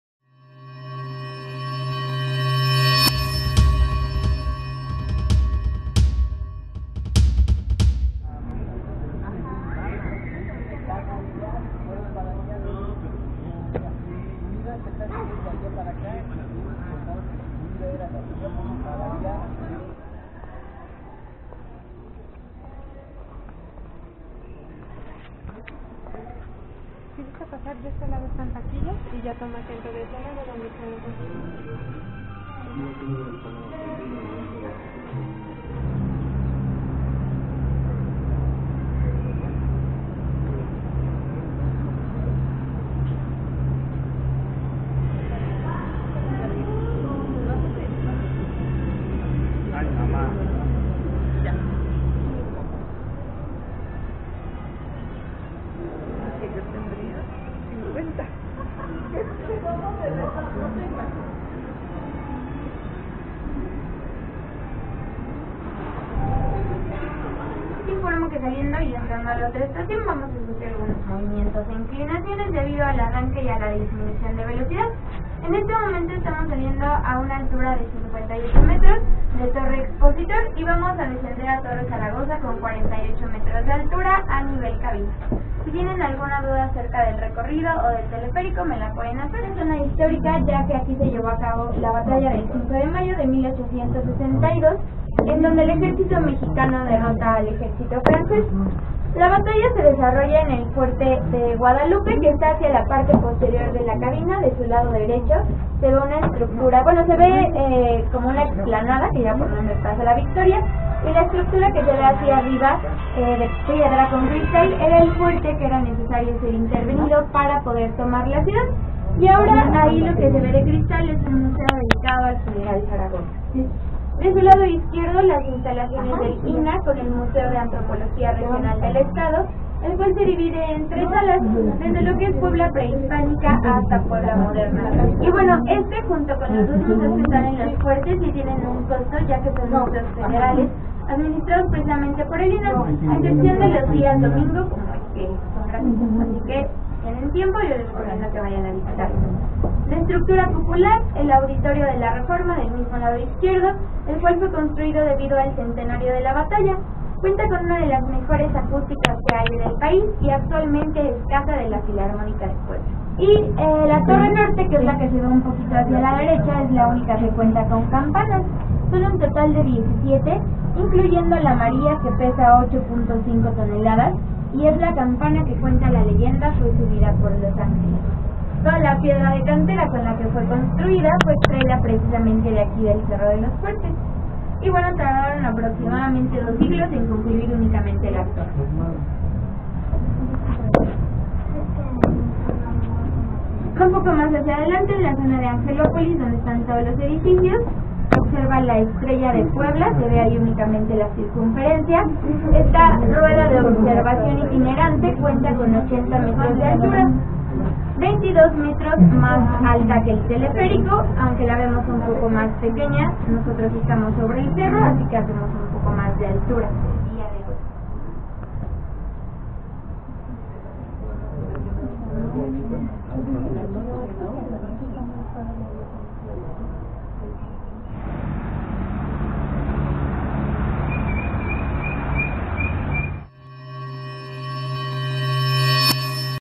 Recorrido del Teleférico de Puebla
Te presentamos parte de lo que puedes ver en el recorrido del Teleférico de Puebla, abierto de martes a domingo con un costo de 30 pesos viaje de ida y 50 pesos viaje redondo.